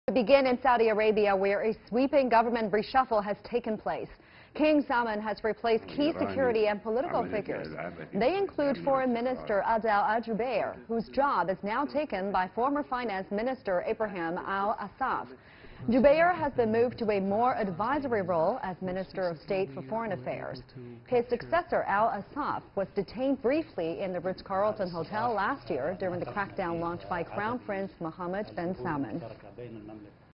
Saudi King orders governemnt cabinet reshuffle
King Salman of Saudi Arabia has replaced key security and political figures. They include Foreign Minister Adel al-Jubeir whose job is now taken by former finance minister Ibrahim al-Assaf. Jubeir has been moved to a more advisory role as Minister of State for Foreign Affairs. His successor al-Assaf was detained briefly in the Ritz-Carlton Hotel last year during the crackdown launched by Crown Prince Mohammad bin Salman.